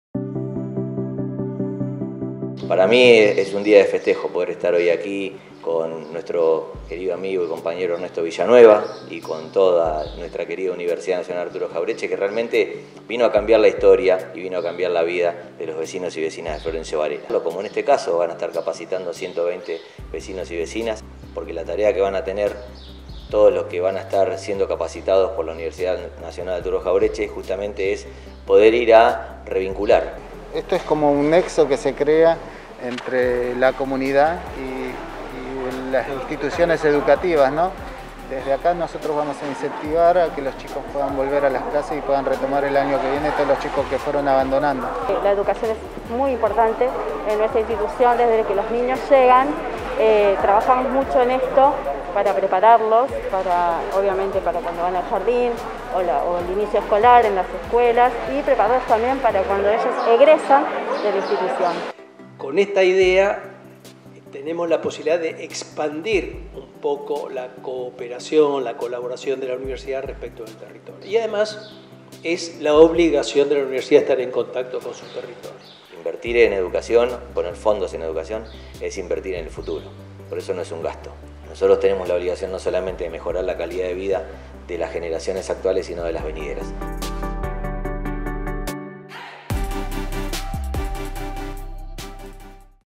El Municipio y la UNAJ rubricaron acuerdo para formar alfabetizadores comunitarios
El intendente -Andrés Watson- junto al rector de la Universidad Nacional “Arturo Jauretche” -Ernesto Villanueva- firmó un convenio para formalizar un plan de capacitación destinado a 120 vecinas y vecinos instruidos en la transmisión de habilidades de lecto-escritura en niños, niñas más adultas esta mañana en la Sociedad de Fomento "El Progreso".
En el establecimiento del barrio Sarmiento, el Alcalde consideró “clave” la preparación de los y las participantes quienes “emprendieron la responsabilidad de propiciar herramientas indispensables bajo la premisa de re-vincular a varelenses con sus trayectorias educativas”.

Posteriormente, el titular en el Ejecutivo local resaltó el trabajo de la Casa de Altos Estudios para “materializar diversas estrategias conjuntas vinculadas con la comunidad. Sin dudas, fue una jornada de celebración que implicó un paso más en nuestra tarea mancomunada”. “Mantuvimos la firme convicción de situar a los procesos pedagógicos como pilares fundamentales en la gestión municipal”, cerró.

En tanto, Villanueva manifestó que el programa fue una muestra más de “la relación estrecha que tuvimos con los distintos sectores del territorio”, al tiempo que resaltó: “Siempre conservó un significado especial concretar proyectos en sintonía con Andrés”.

El presidente de la entidad anfitriona -Manuel Salto- definió la presentación de la propuesta como “una estrategia para potenciar la labor de las instituciones que desplegaron su agenda pragmática en los barrios”. Además, previó “un impacto positivo en la ciudadanía por contribuir en la escolarización de los chicos y las chicas”. “Definitivamente, una manera de afianzar el nexo de la sociedad con los establecimientos educativos e impulsar a la reinserción estudiantil”, aseguró.

La coordinadora en el Hogar de Niños “Arco Iris”, Karina Romero, exhibió su conformidad por ser partícipe de una iniciativa para “transformar la vida de las personas”. “La enseñanza resultó un factor determinante en las instancias de aprendizaje de las próximas generaciones. Trabajamos para acompañarlos desde su ingreso al nivel inicial hasta su egreso”, describió.

Además, el Mandatario y la máxima autoridad en la UNAJ concretaron la entrega de kits escolares destinados a más de 20 espacios comunitarios: lápices, lapiceras, cuadernos, sacapuntas, entre otros útiles.

Estuvieron presentes la secretaria de Educación municipal, Andrea Digiobani; el jefe distrital de Educación, Alfredo Schveizer; el inspector de Región Cuarta de DIEGEP, Leonardo Gómez; y la presidenta del Consejo Escolar, Claudia Allerbon.